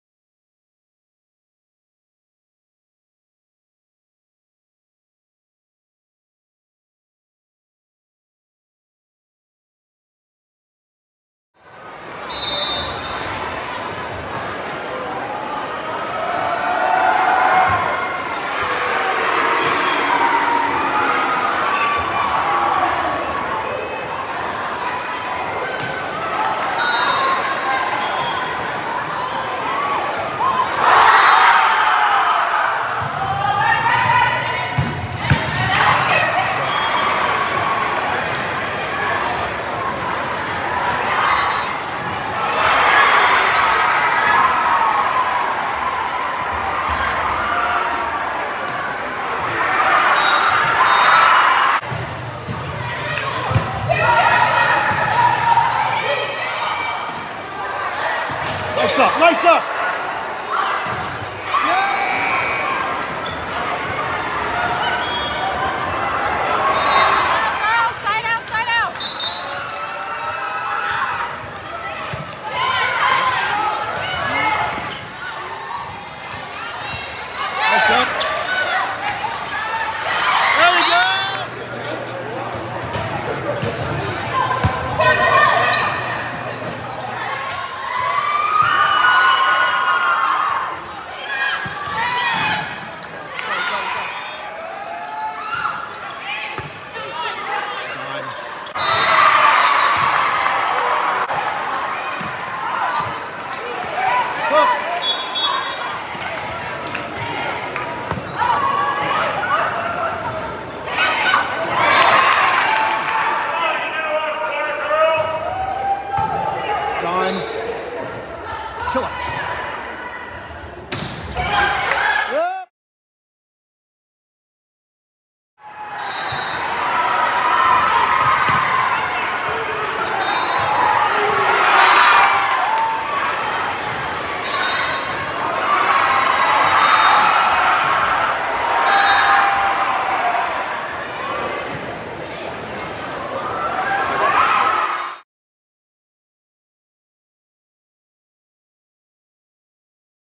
Brooke Whitlock Volleyball Highlights
Brooke Whitlock
Utah HIVE Volleyball 17U Gold Travel Team
Weber High School
5’10”
3.9 GPA
OH/DS